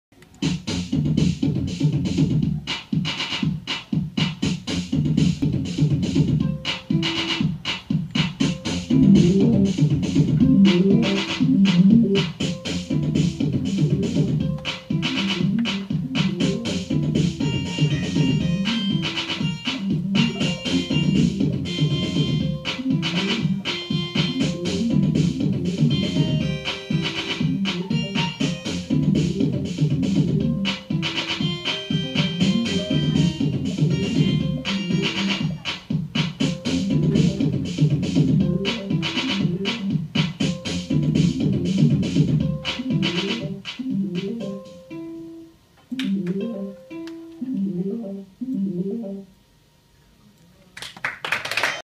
Conductive Music - Noreen Sher
Last week ‘Conductive Music’ returned for the second and final stage of workshops in which Year 10 Music and Textiles students customised an item of clothing with open-source wearable hardware, conductive thread, and e-textiles before creating a solo multimedia performance! The students performed their wearable instruments live along to backing tracks they had created earlier in the day. Well done to all the Year 10 students who took part for their imaginative and creative work and a huge thanks to the Conductive Music team.